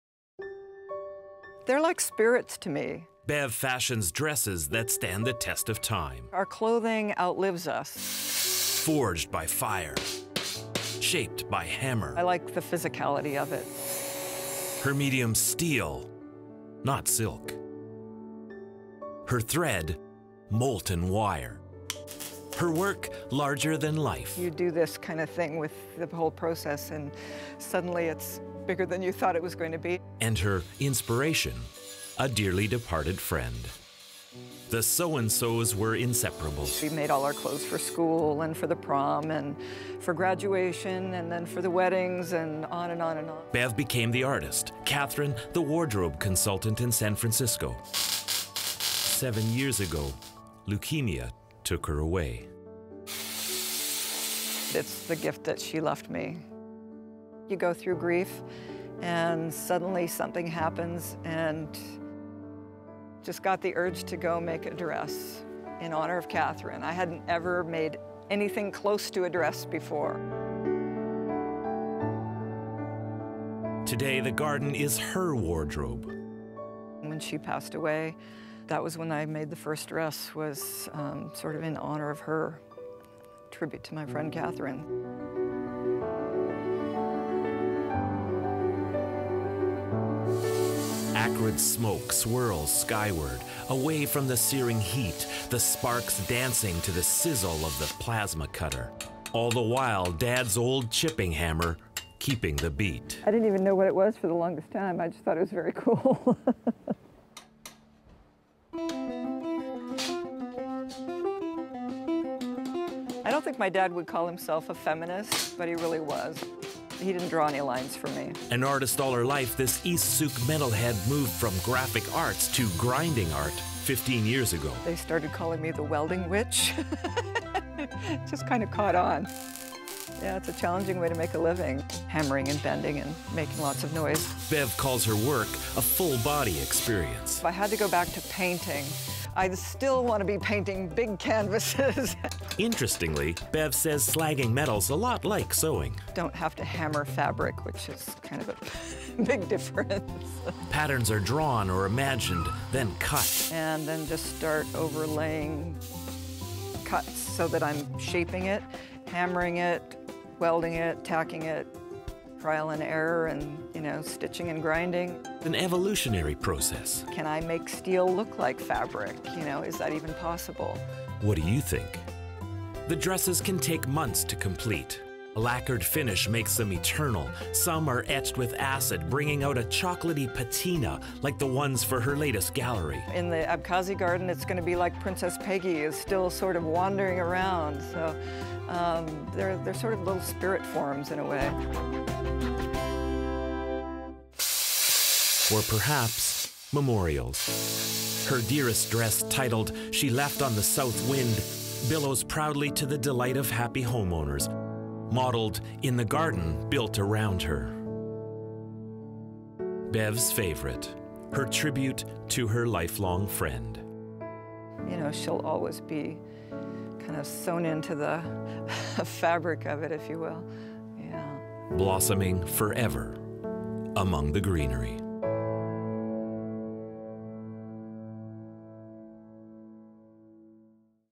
Dresses that stand on their own. Meet metal artist making magic!
Shaw TV's Paul Beilstein visits a dressmaker turned artist who's passion is working with heavy metal. And her inspiration:  her best friend.